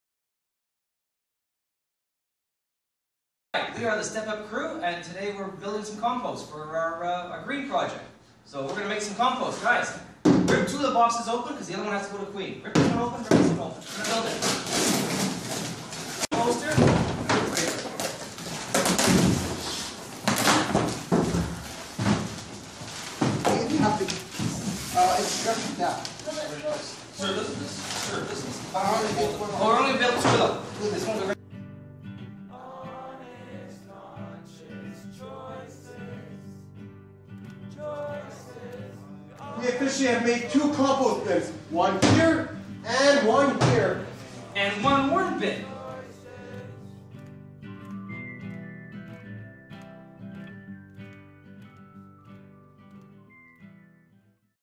Building Our Compost Bins
Our Step-Up students building compost bins.